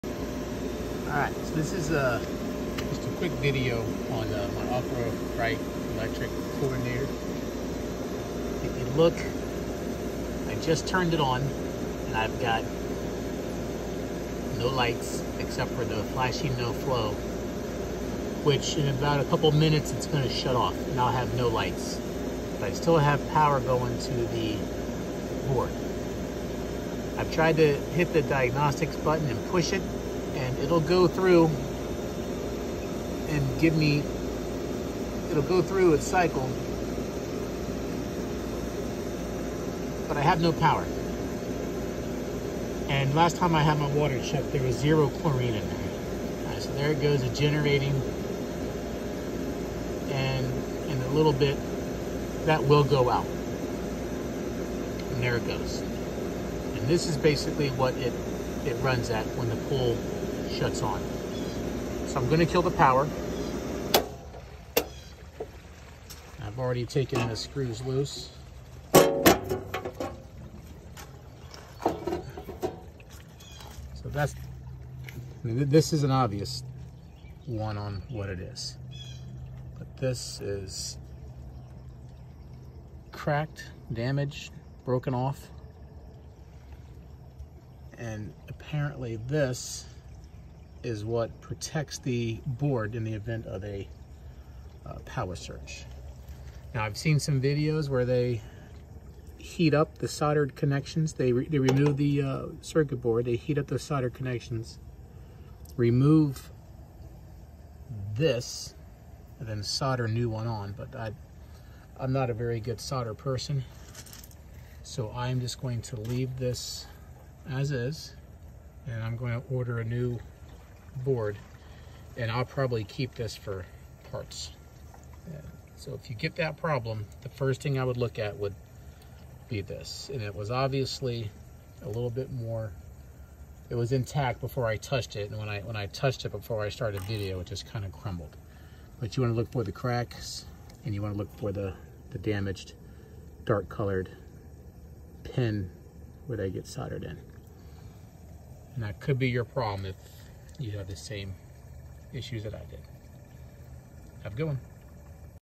Aqua Rite no power no lights
I noticed I had no lights on my chlorinator. After some research I found the problem is the thermistor. I did end up buying a two pack off Amazon for 10 bucks and will solder one in instead of paying 400 dollars for a new one